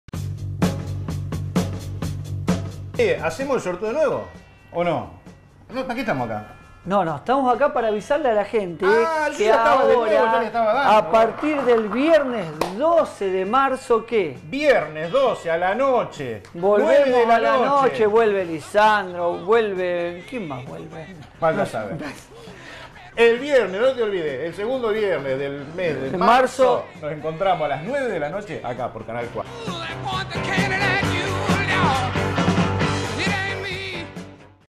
BINGO SOLIDARIO 2021 de los Bomberos Voluntarios roldanenses
Todas las semanas, el gran sorteo del BINGO SOLIDARIO de la Asociación Bomberos Voluntarios de Roldán, con el objeto de continuar solventando su invaluable función en nuestra sociedad.

Nuevo día y horario: todos los viernes, desde las 21 horas.